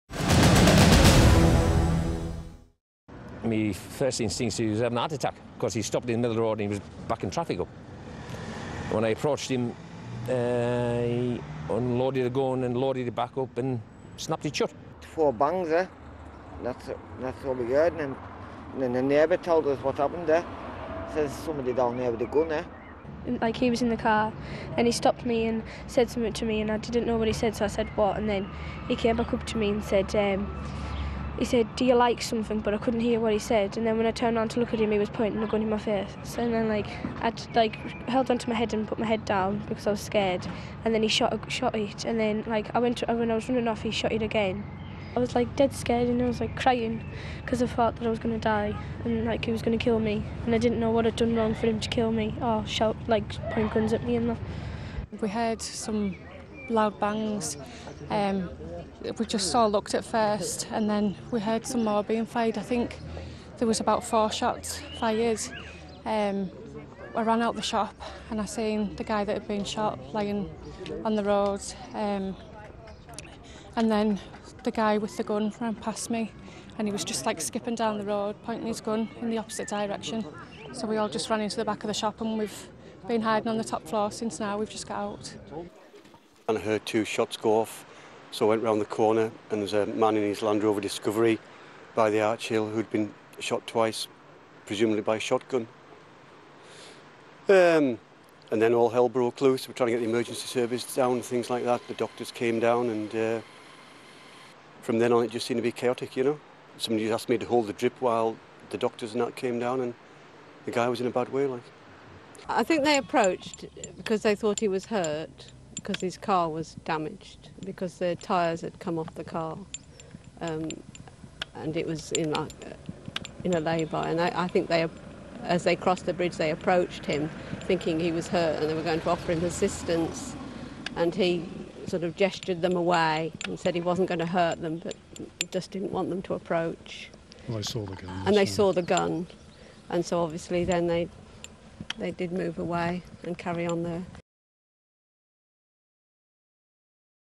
Eyewitnesses describe Cumbria shootings
Eyewitnesses have been describing their horror as gunman Derrick bird went on the rampage killing 12 people.